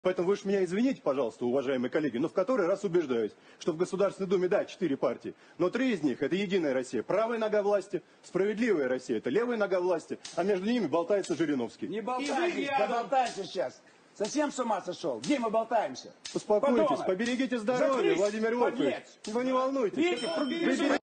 КОММУНИСТЫ НАЗВАЛИ ЖИРИНОВСКОГО ХУЙЛОМ
Коммунист подорвал Жирика анекдотом дня на дебатах.

Клоуна одолели его же оружием.